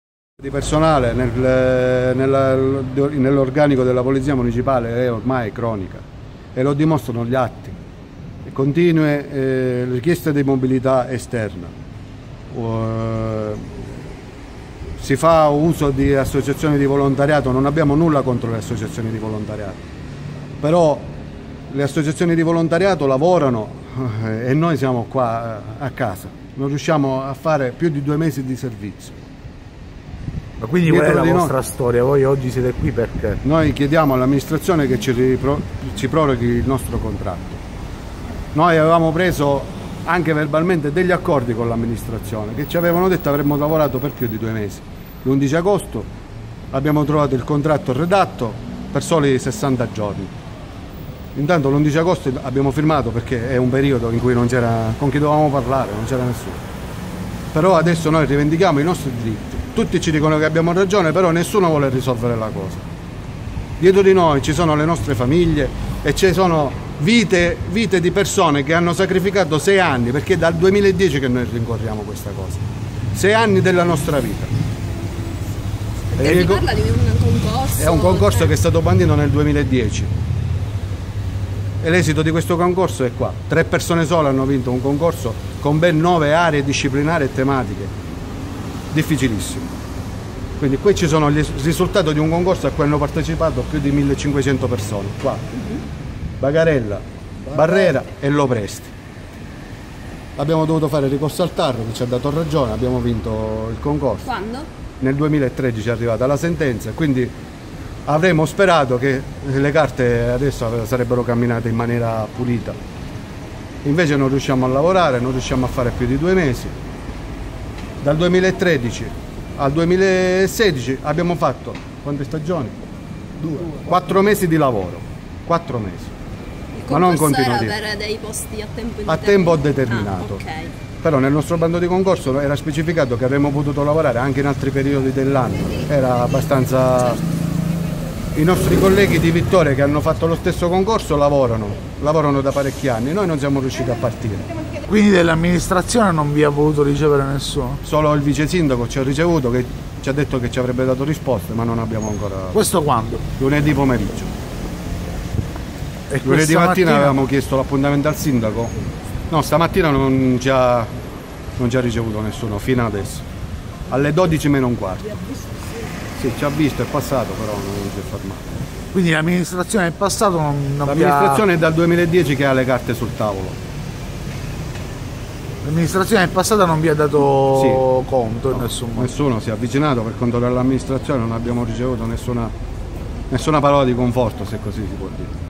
"Vigili usa e getta" si incatenano al Municipio di Ragusa
Tre aspiranti vigili urbani si sono incatenati davanti a Palazzo dell'Aquila a Ragusa: sono vincitori di concorso per dei posti a tempo determinato, ma non li fanno lavorare.